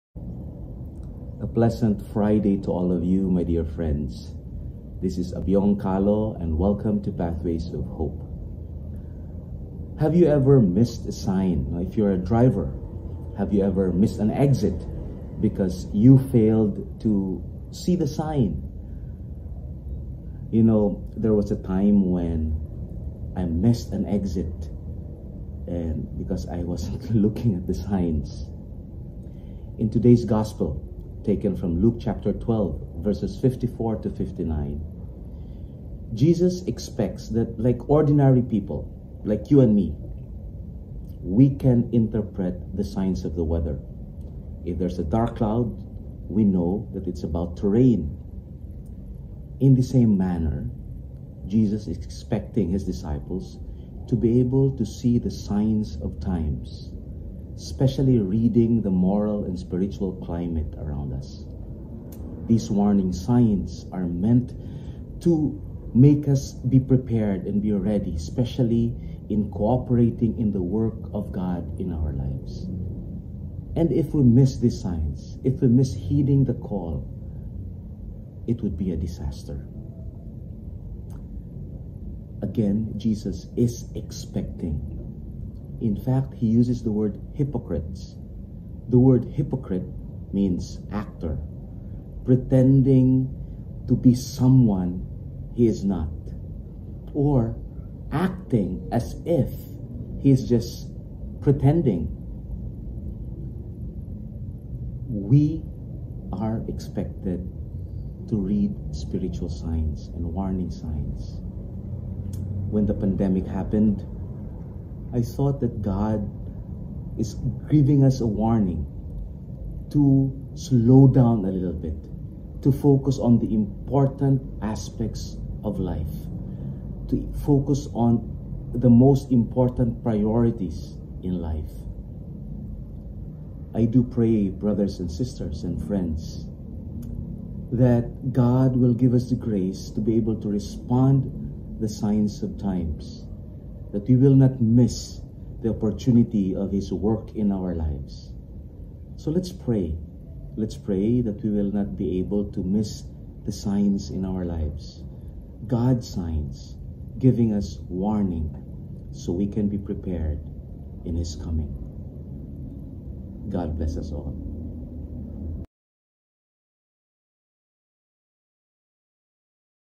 PATHWAYS OF HOPE : “Heed the Warning Signs”
Friday of the 29th Sunday in Ordinary Time
Memorial of Saint Hilarion

[GOSPEL OF THE DAY]   LUKE 12: 54 - 59

Jesus said to the crowds,
“When you see a cloud rising in the west
you say immediately that it is going to rain–and so it does;
and when you notice that the wind is blowing from the south
you say that it is going to be hot–and so it is.
You hypocrites!
You know how to interpret the appearance of the earth and the sky;
why do you not know how to interpret the present time?
“Why do you not judge for yourselves what is right?
If you are to go with your opponent before a magistrate,
make an effort to settle the matter on the way;
otherwise your opponent will turn you over to the judge,
and the judge hand you over to the constable,
and the constable throw you into prison.
I say to you, you will not be released
until you have paid the last penny.”

Video by Abyong Calo : Licensed Electrical Engineer. Lay missionary brother: The Servants of the Word. Regional Director for Mega Manila and International missions (UAE and Singapore), Ang Lingkod ng Panginoon.